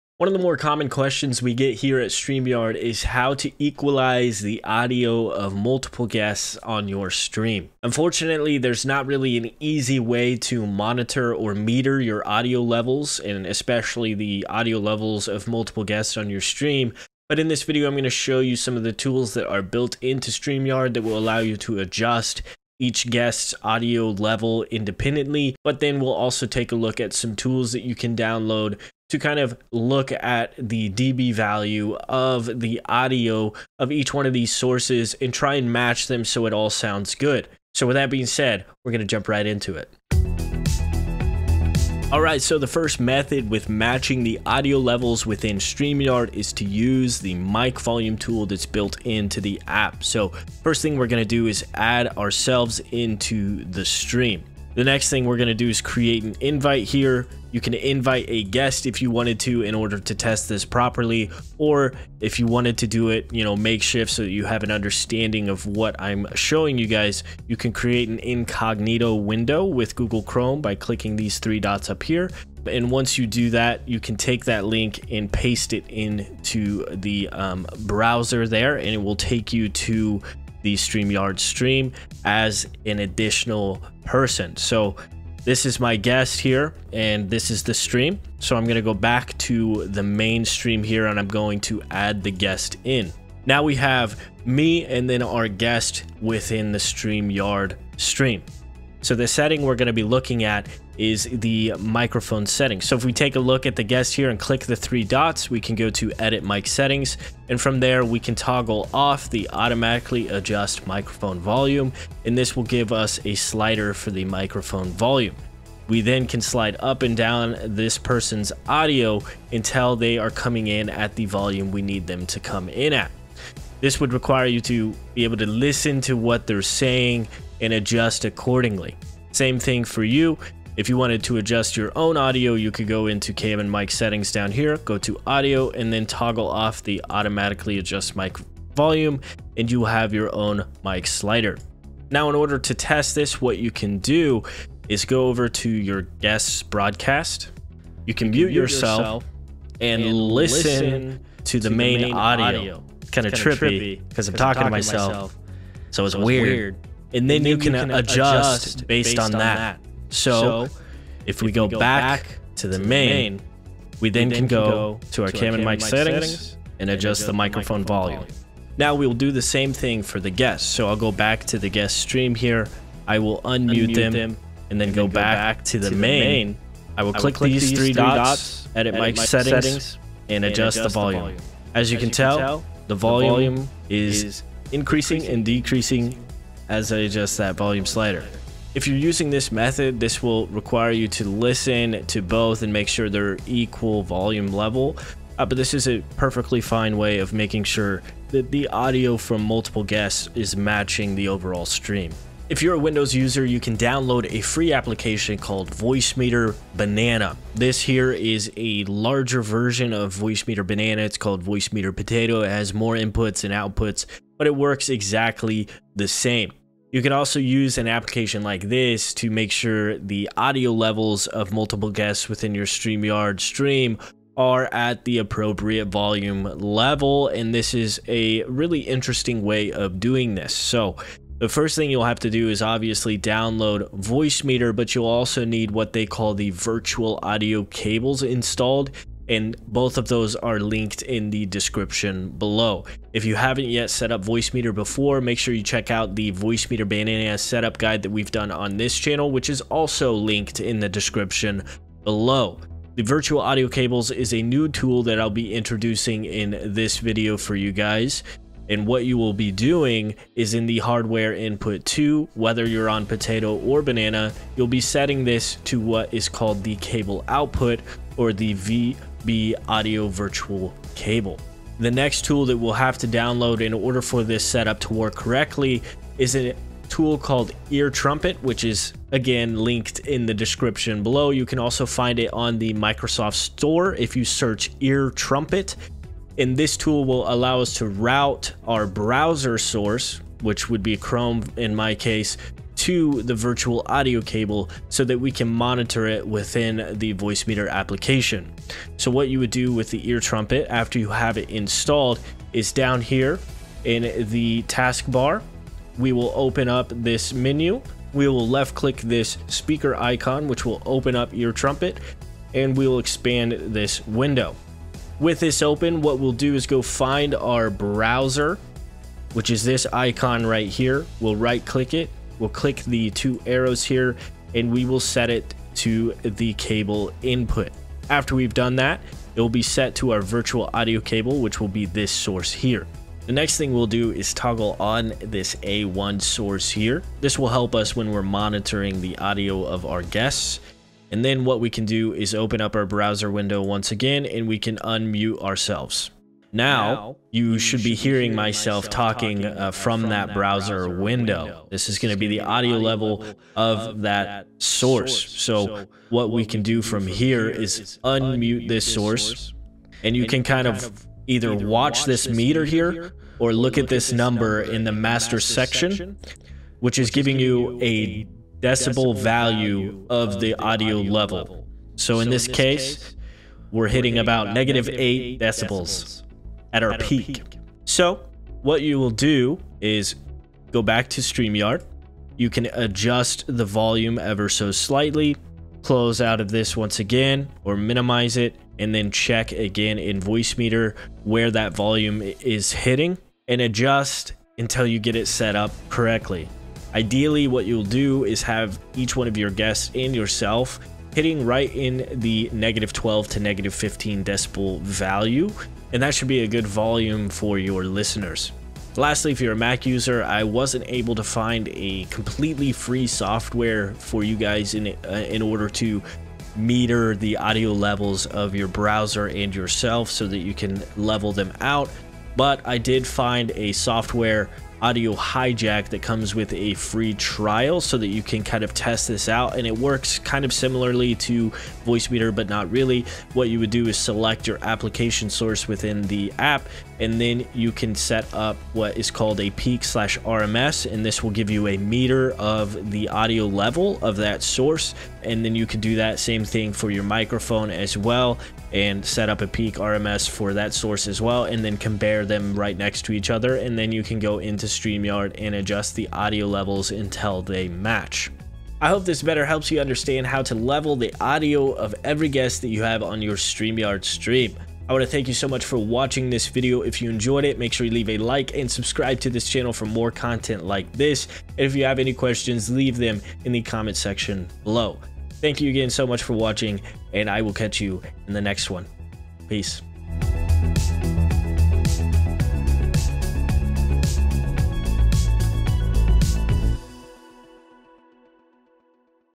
How to Do Audio Leveling When Live Streaming
Learn how to level audio when live streaming to help match the audio between you and your guests.

When you have uneven audio, one person is louder than your guests.

In this StreamYard tutorial with TheFranswah Studio, you will learn about different ways that you can level audio for your guests for a better-sounding live stream.